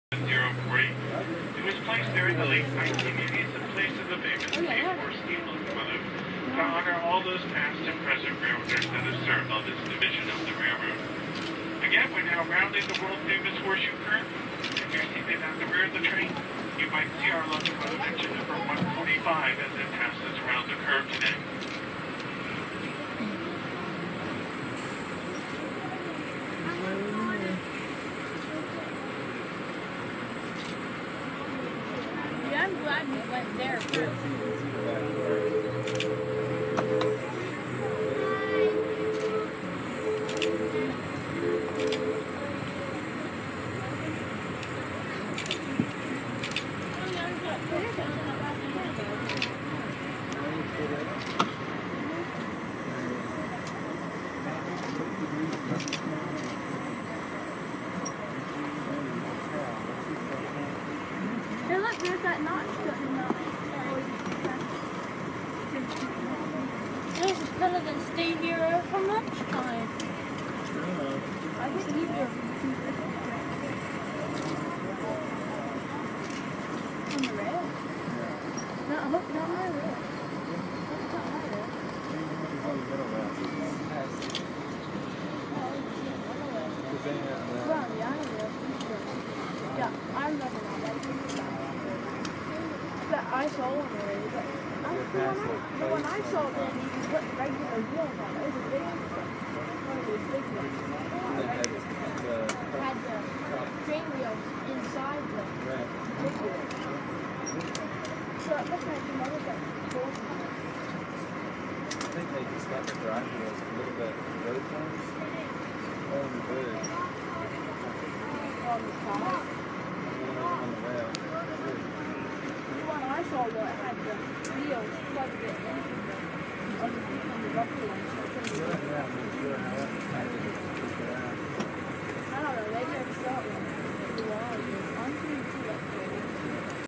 Westbound Pennsylvanian at Horseshoe Curve
Amtrak train 43 makes the 120 vertical climb up Horseshoe Curve. The Pennsylvanian is running 28 minutes late out of Altoona.